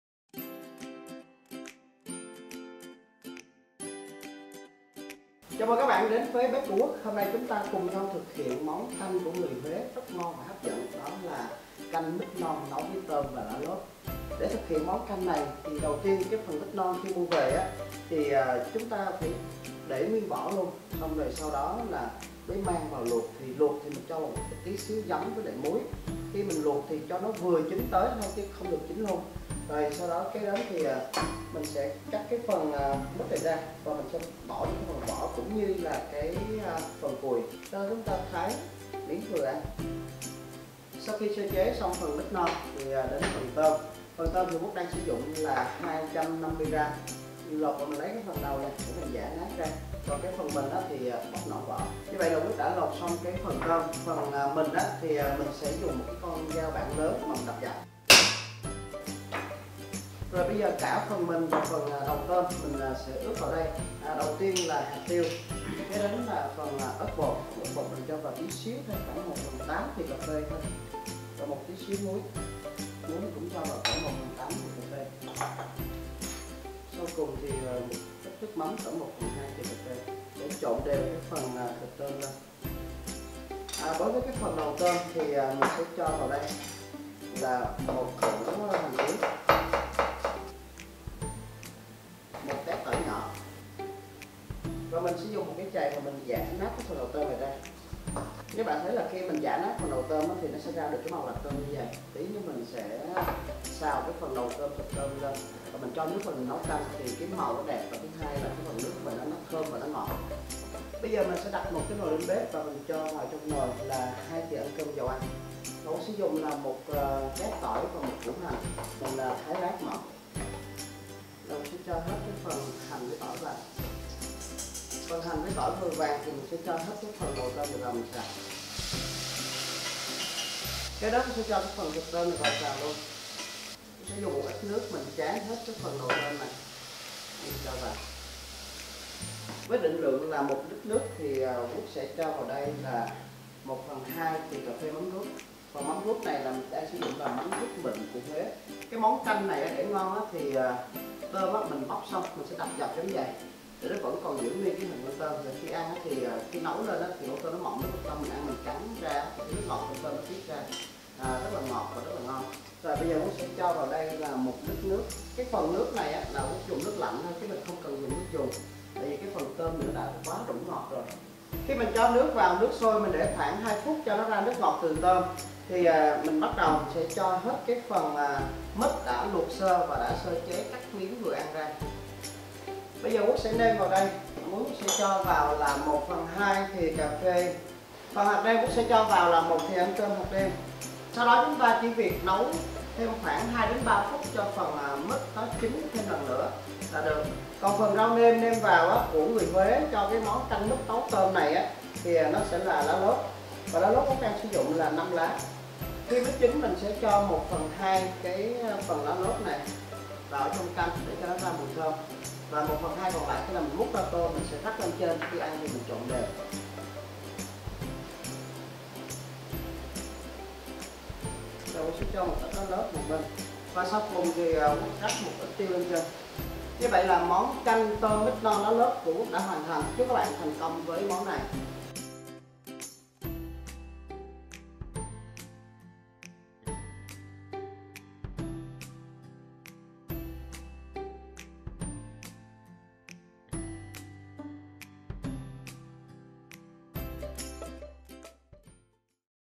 Cách nấu Canh Mít Non Tôm Thịt cực hấp dẫn l Võ Quốc l Món Ngon Việt Nam
Canh mít non tôm thịt nấu lá lốt là món canh của người Huế rất ngon và hấp dẫn.
500g mít non còn vỏ ( 500g unriped jackfruit with skin )
200g tôm đất ( 200g shrimp )
1 thìa cà phê mắm ruốc ( 1 coffee spoon shrimp paste )
1 thìa cà phê ớt bột ( 1 coffee spoon chilli powder )
5 lá lốt ( 5 piper lolot )
1/2 thìa cà phê muối ( ½ coffee spoon salt )
1 thìa súp hạt nêm  ( 1 tbs broth mix )
2 thìa súp dầu ăn ( 2 tbs oil )
© Bản quyền thuộc Ezi Cooking / Món Ngon Việt Nam
© Copyright by Ezi Cooking / Món Ngon Việt Nam ☞ Do not Reup